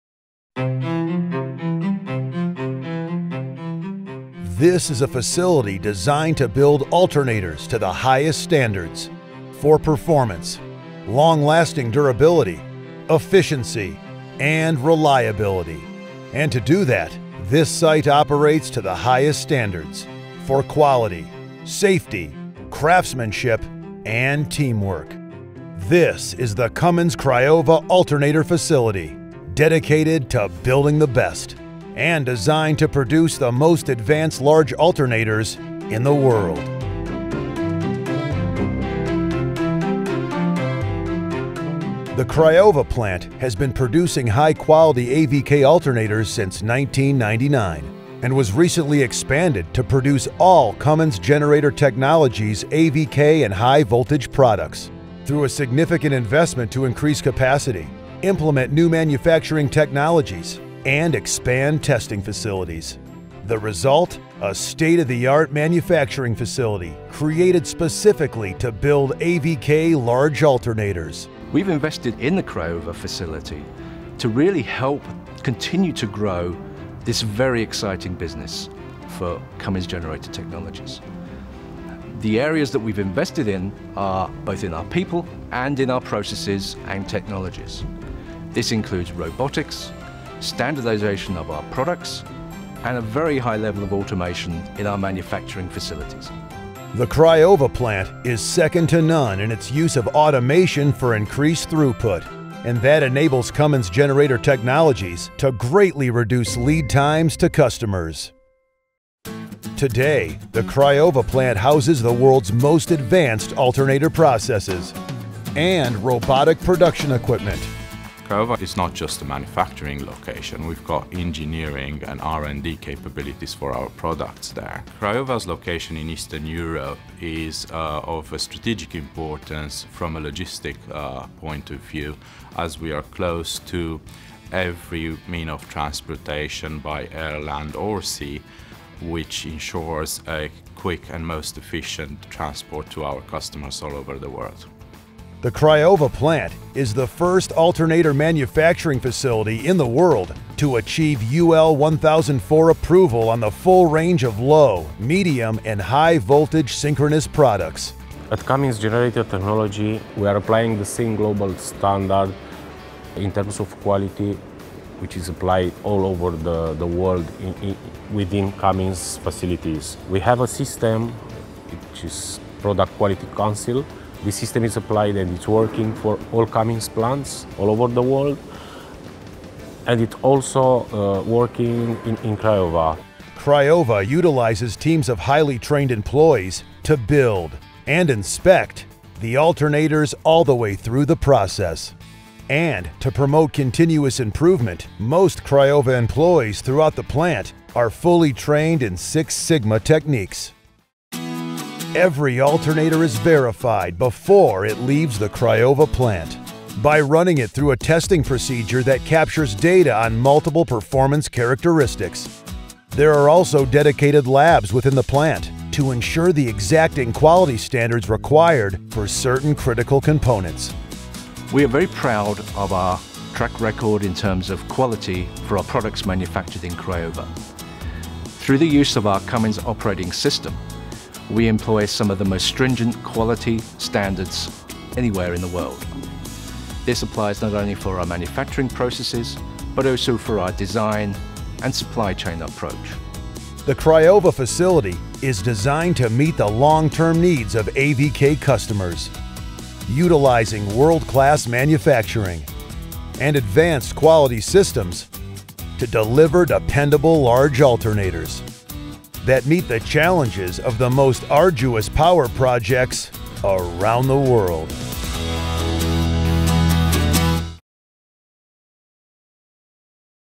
Introducing Craiova: A Centre of Excellence for Large Alternators
A wholly-owned Cummins facility, the Craiova plant has been manufacturing rugged and reliable AvK® alternators since 2000. Recent capital investment ensures our customers benefit from increased capacity, the implementation of new manufacturing technologies, sophisticated testing facilities and reduced lead-times.

The result is a state-of-the-art manufacturing facility, specifically to build AvK® large alternators.